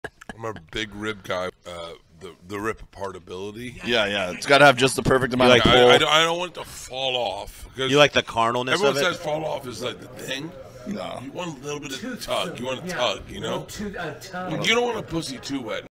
Bert Kreischer tugging on meat
Bert Kreischer, Ryan Reiss, Shane Torres, Dave Williamson, Kitty Reynolds, Mehran Khaghani, Tere Bernace talk BBQ  at the world famous comedy cellar.